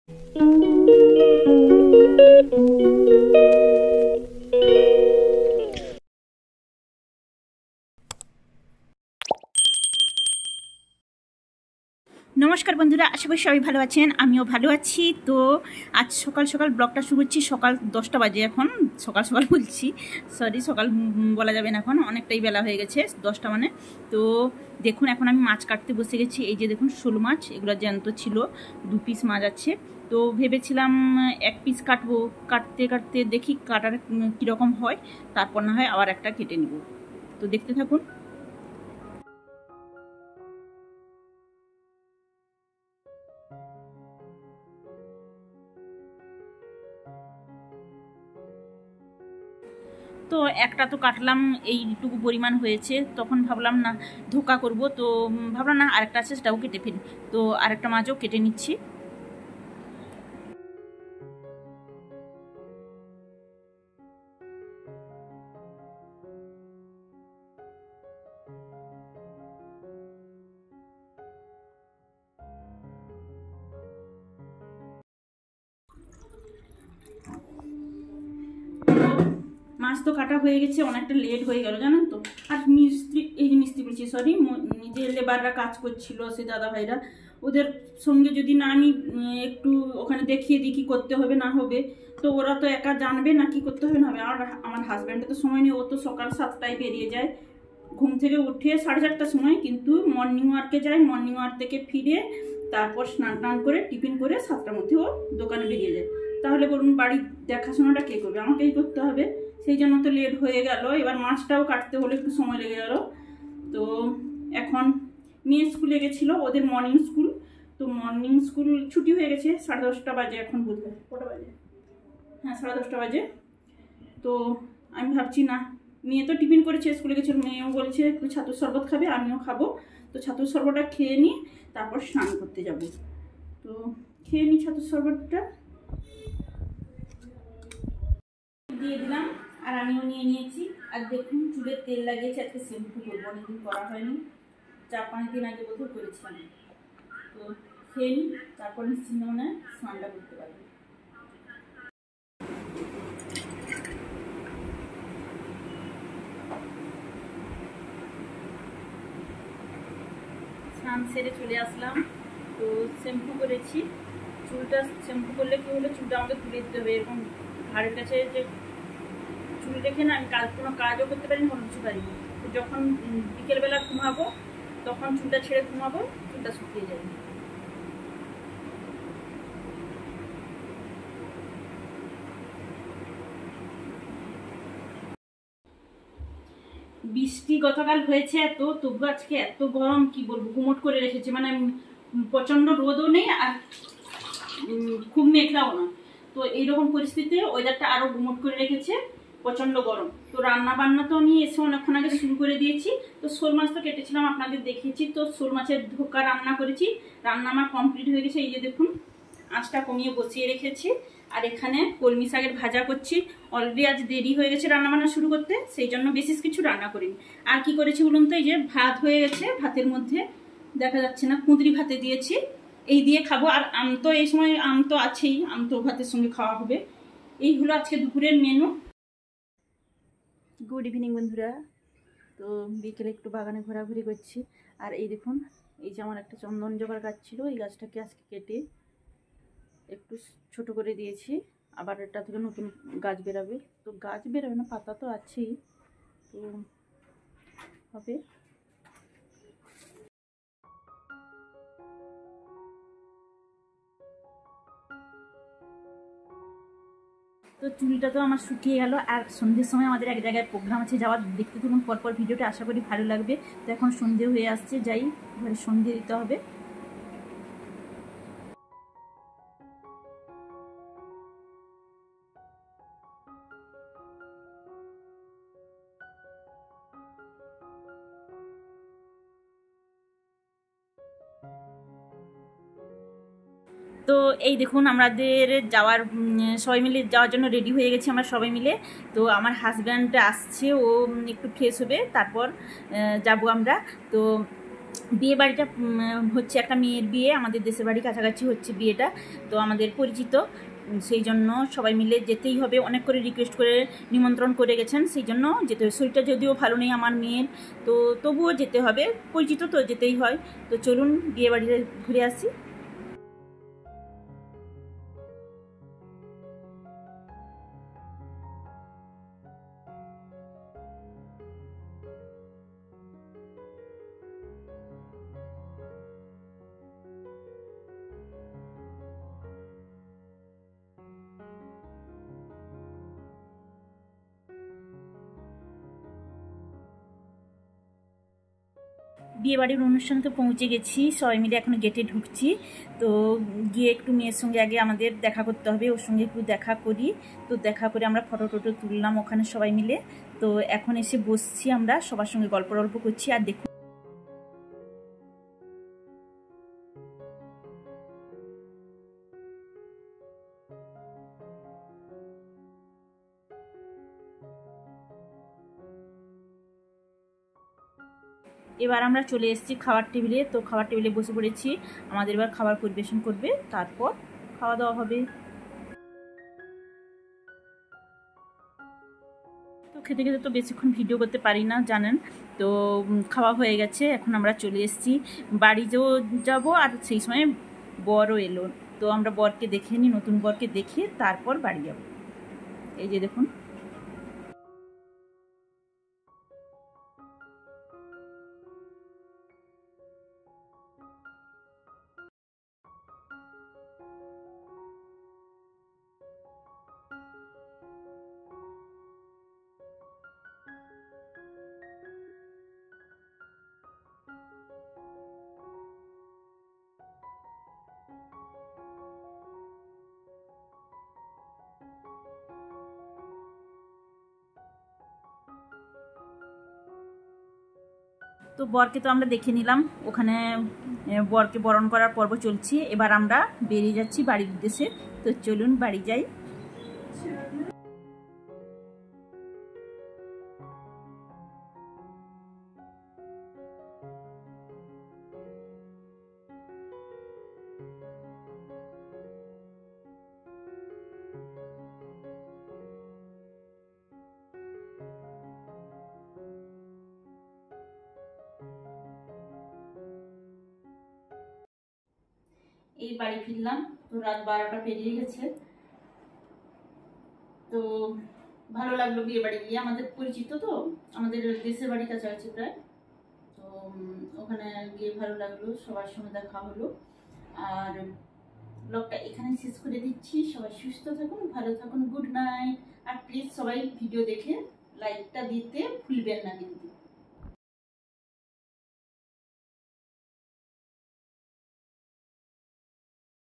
জোড়া শোল মাছ কেটে ধোকা রান্না করলাম আর সন্ধ্যের সময় বিয়ে বাড়ির অনুষ্ঠানে গেলাম।
Maitri Bengali Vlogs